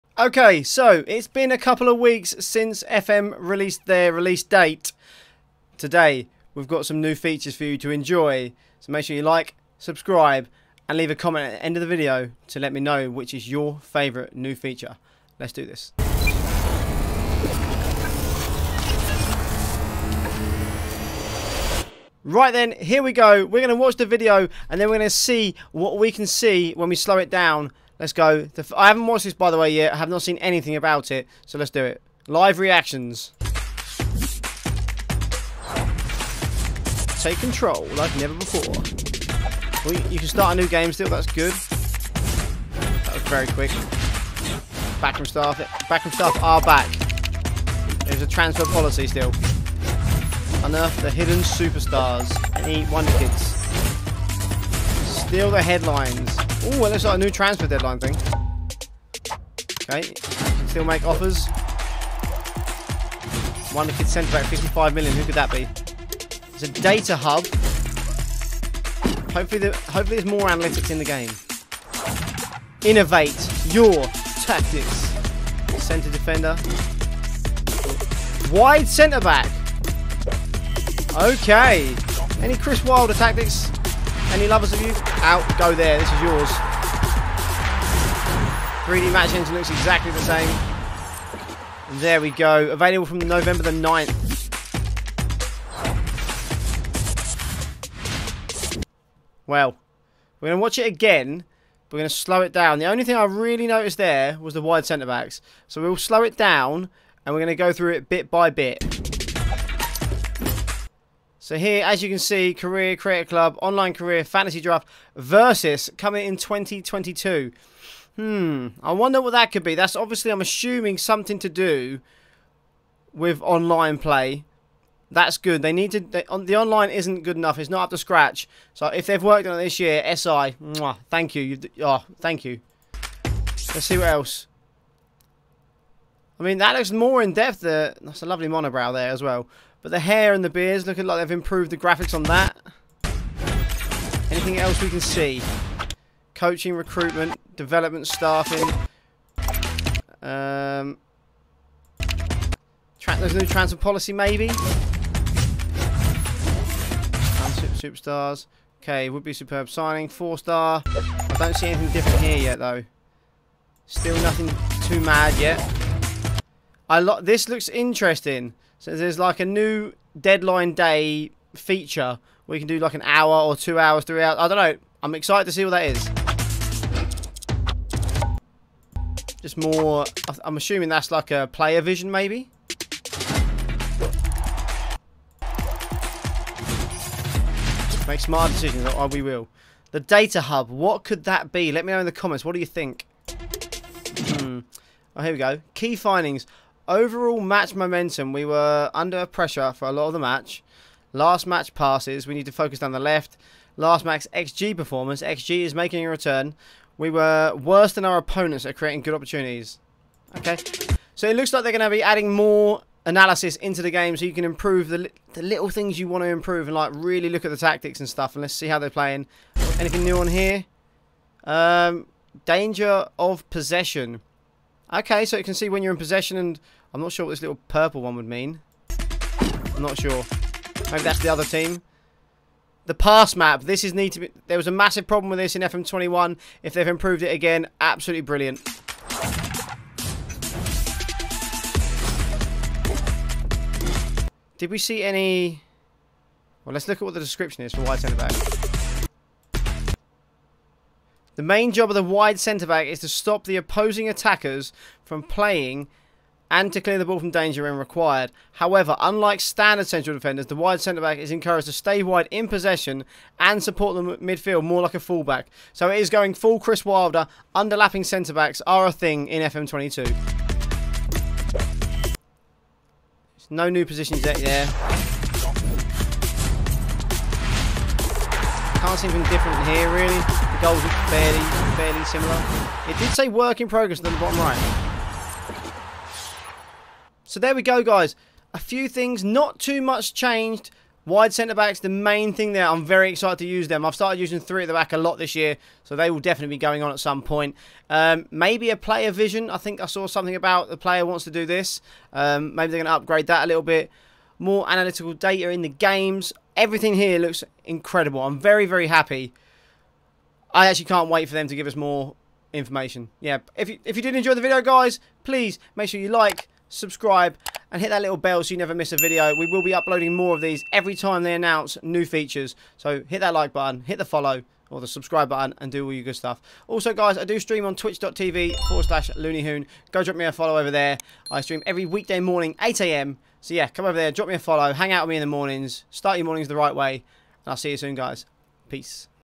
FOOTBALL MANAGER 2022 NEW FEATURES!!
Football Manager 2022 has dropped some new features! Let's find out what we have to look forward to!